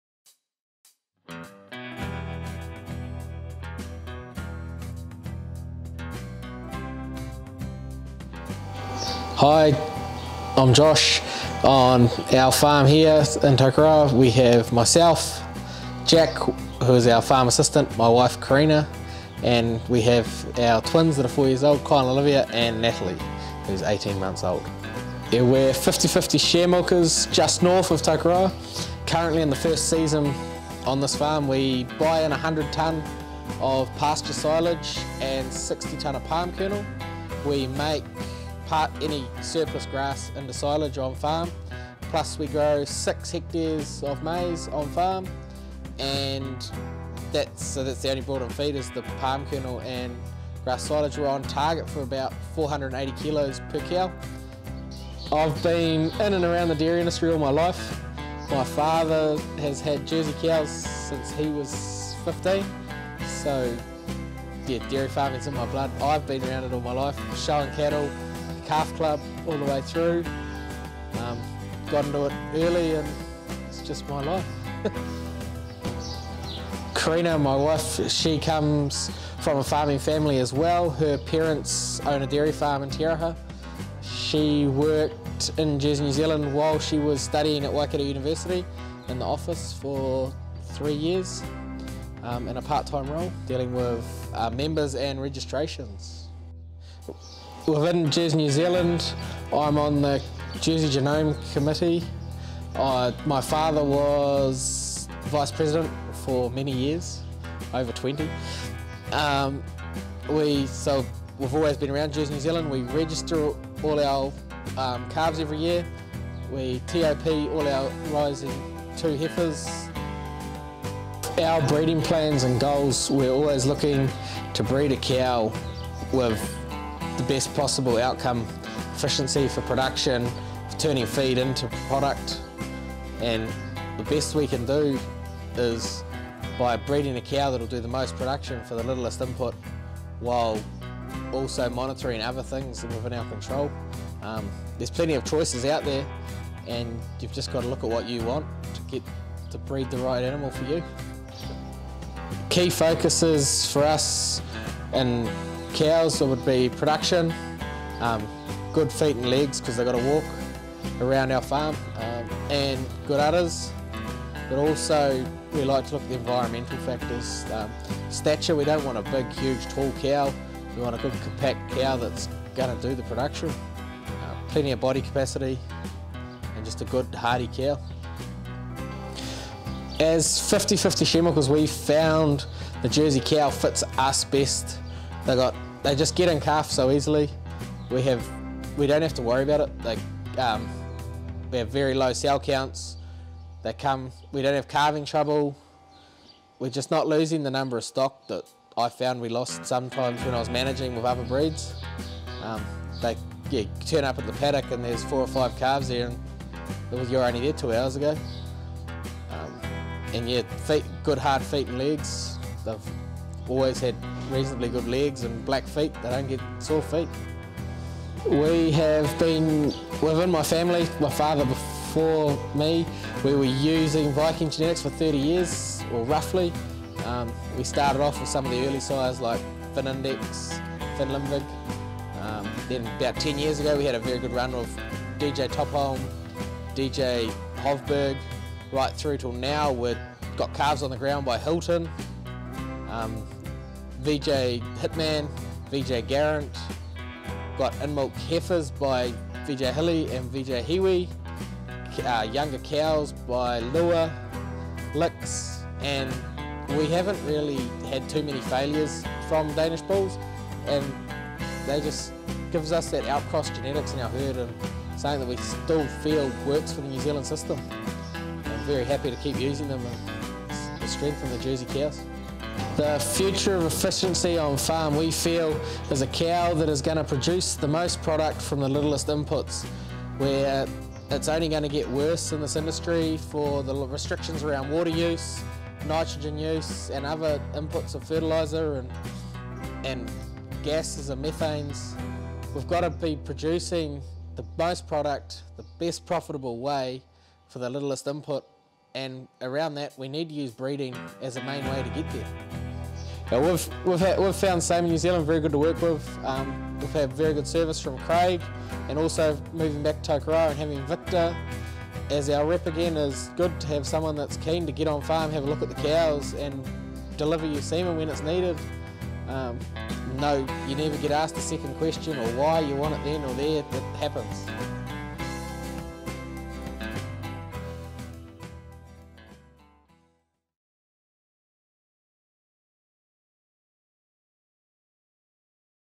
Tahau Jerseys and their 30 year VikingJersey experience.
Learn about Josh and Kareena Sneddon's dairy journey with the VikingJerseys just north of Tokoroa.